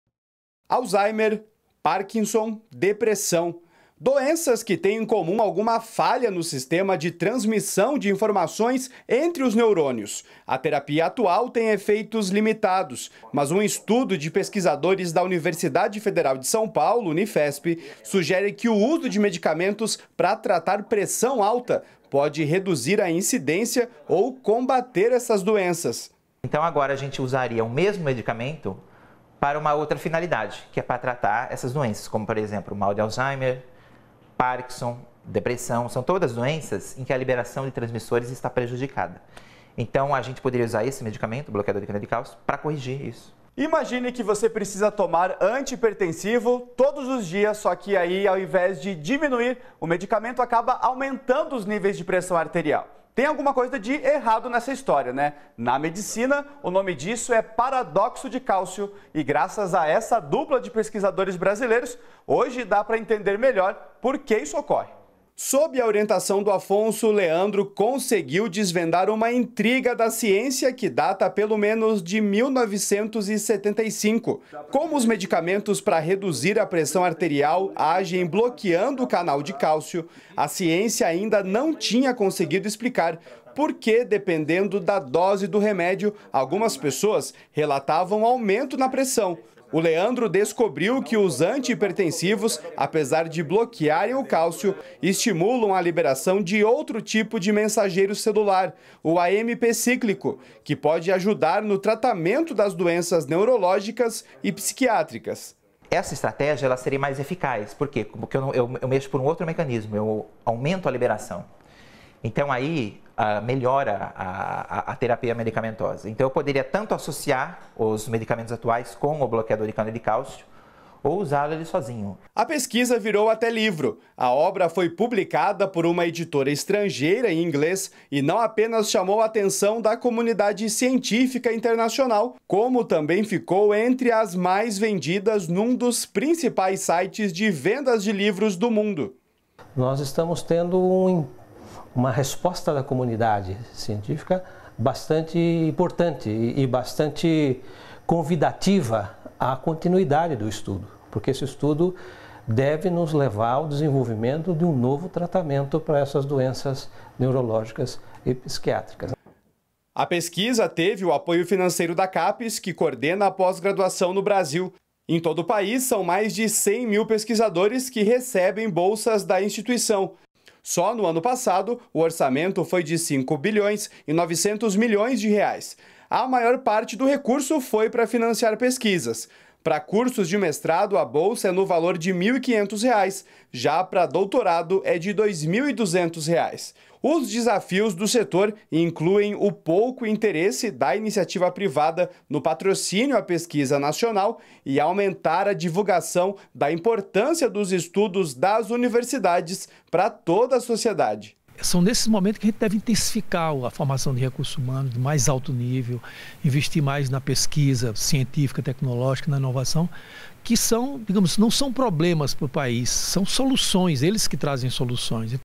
Governo repassa R$ 312,4 milhões para Capes
REPÓRTER NBR - 16.01.17: um estudo de pesquisadores brasileiros pode tornar mais eficaz o tratamento de Alzheimer, Parkinson e Depressão. Pesquisas dos nossos cientistas estão chamando a atenção de outros países. E pra continuar esse trabalho, o Ministério da Educação liberou, neste mês, R$ 312 milhões.